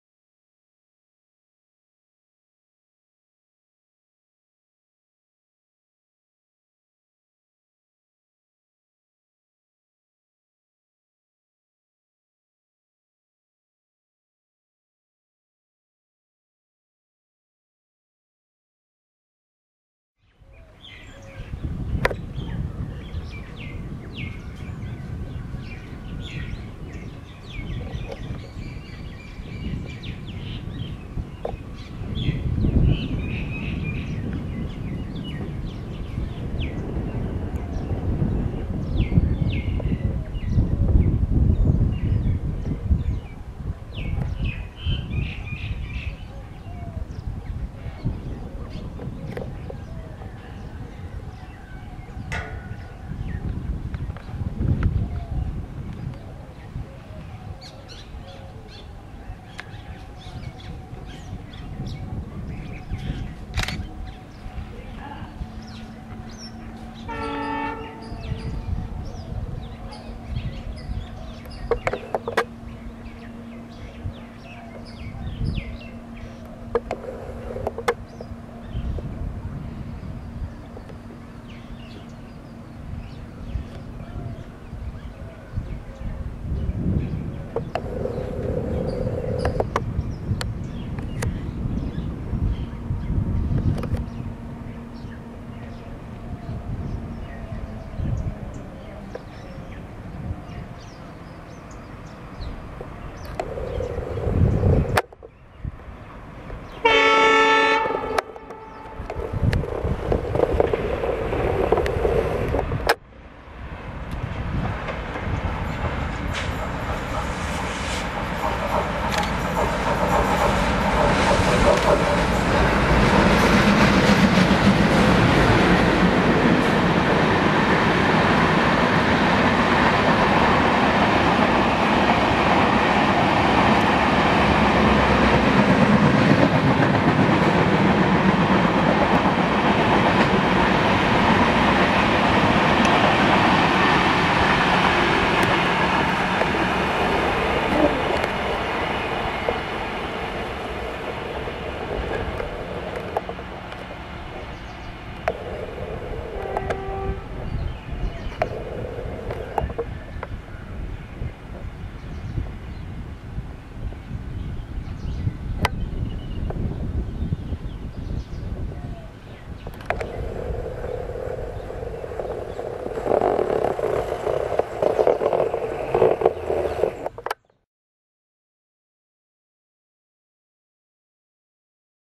ALCo TWINS @ TUNNEL & Station SKIP in STYLE | Pure DIESEL MAGIC | Barmer express | Indian Railways
Comments and suggestions are welcome.